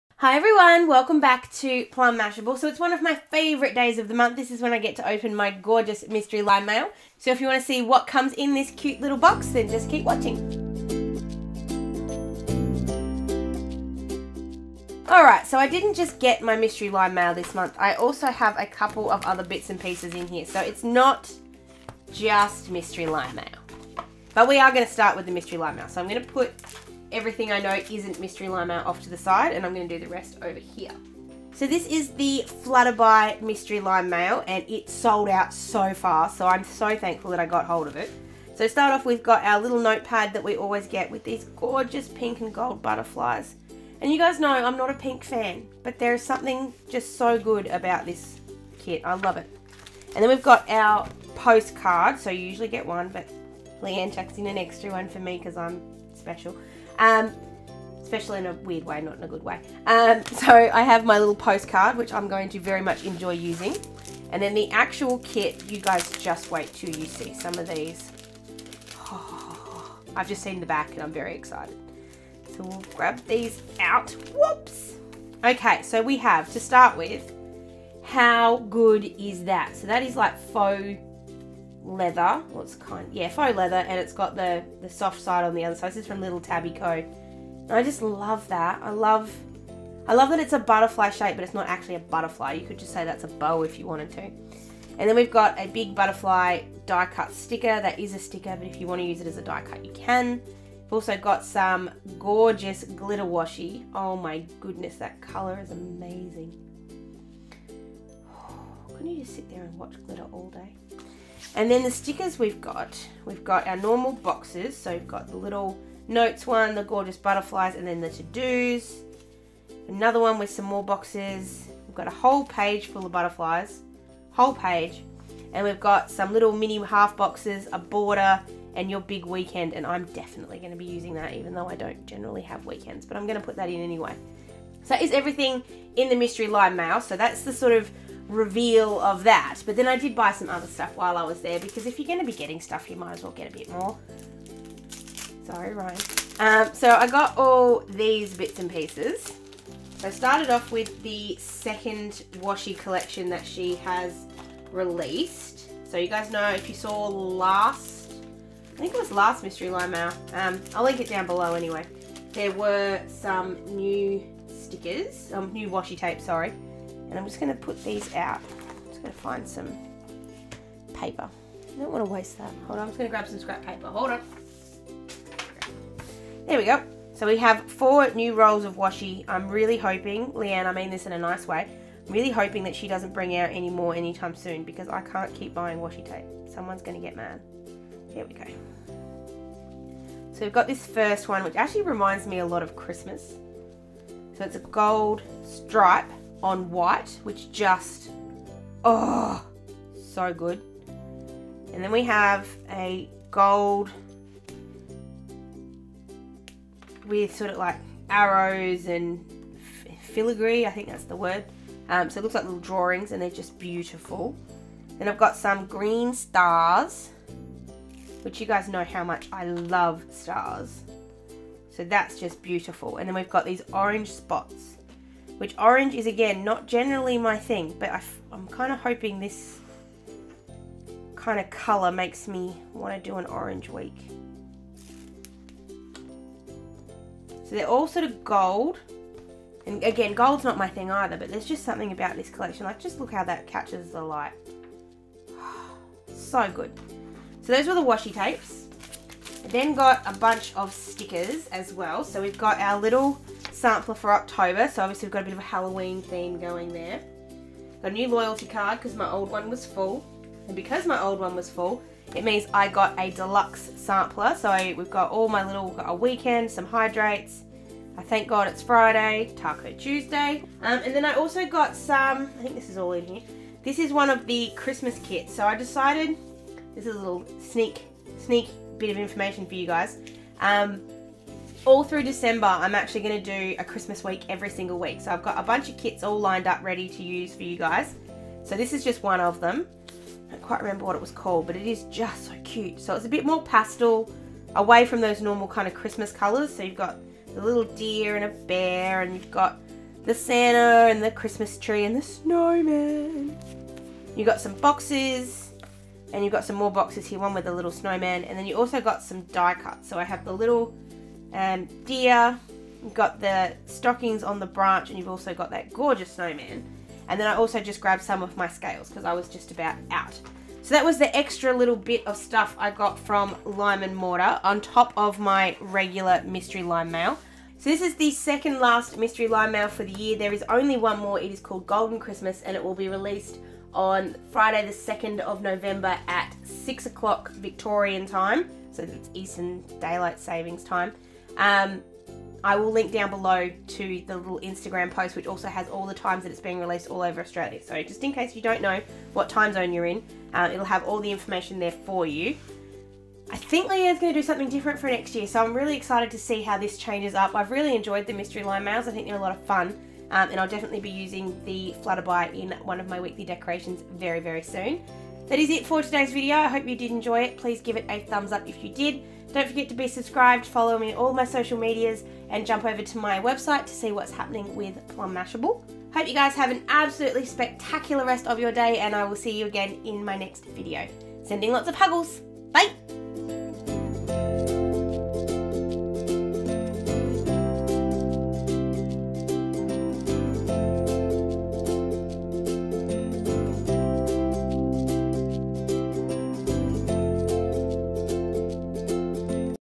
Lime & Mortar Mystery Lime Mail Reveal - October Flutterby + Haul
Lime & Mortar make some of the best stickers on the market, and I love getting the Mystery Lime Mail each month. This month I also grabbed some other items, including a very pretty Christmas kit.

For more info on the next MLM release, check out the Lime and Mortar Instagram

Beehaven Designz: ASHLEIGH10 for 10% purchases (excludes Honey Pot)
VIdeos Featured:
Lime & Mortar Mystery Lime Mail Reveal - September Blue Rose

Products Featured:
Mystery Lime Mail: $22.00

Lime & Mortar Skinny Foil Washi Bundle 2 - $2.50 each or 4 for $9.00

Lime & Mortar Full Box Planner Stickers - Cute Christmas: $5.00

Lime & Mortar Die Cut Stickers - Cute Christmas: $1.00 each